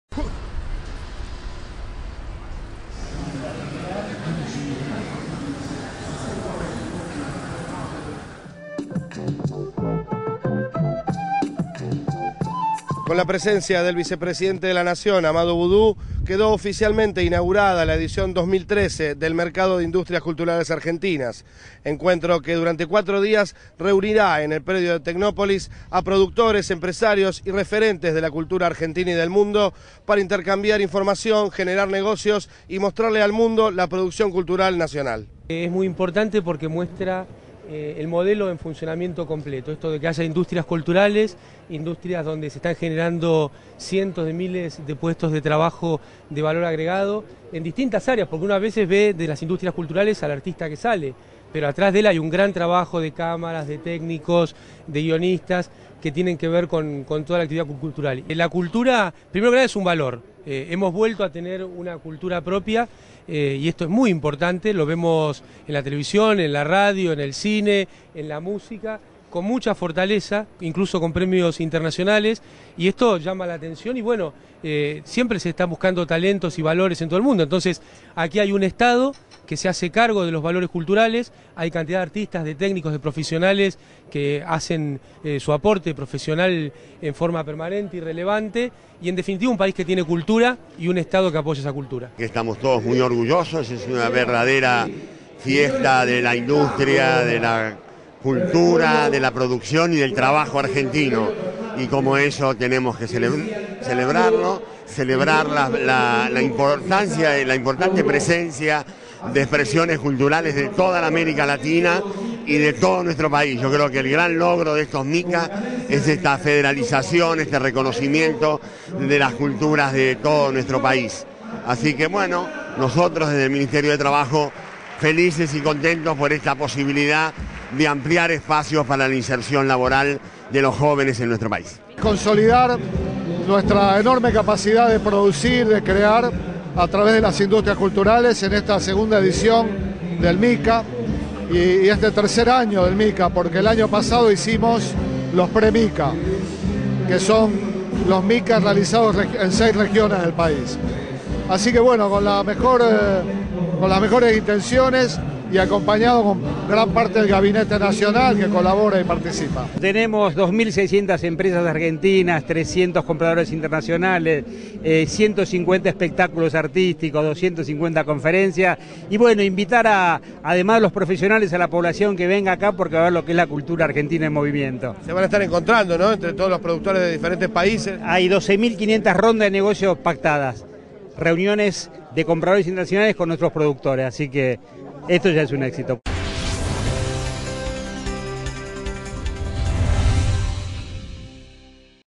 Boudou inauguró el MICA 2013
El vicepresidente de la nación, Amado Boudou, dejó inaugurada la segunda edición del Mercado de Industrias Culturales Argentinas.
El encuentro que durará cuatro días, tiene por objeto, reunir a los productores, empresarios y referentes de las industria de la cultura argentina, con sus pares de países de todo el mundo.